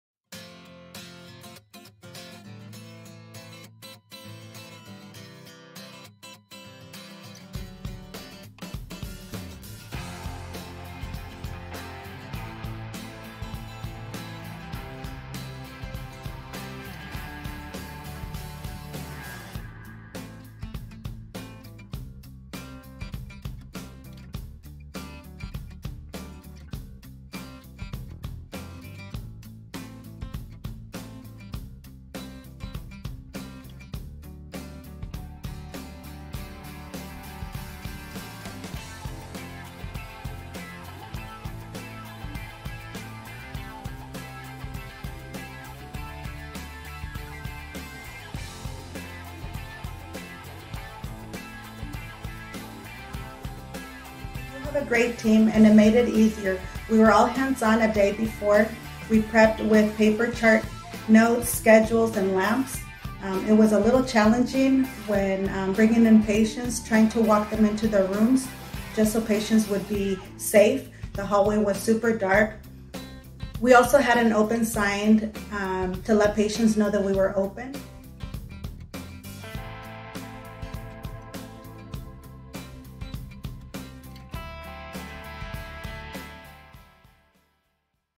Monday Minute - Power Outage
Our OLE Health team pulled together to keep the doors open to care for our patients during a several-days Public Safety Power Shutoff (PSPS) event in October 2019.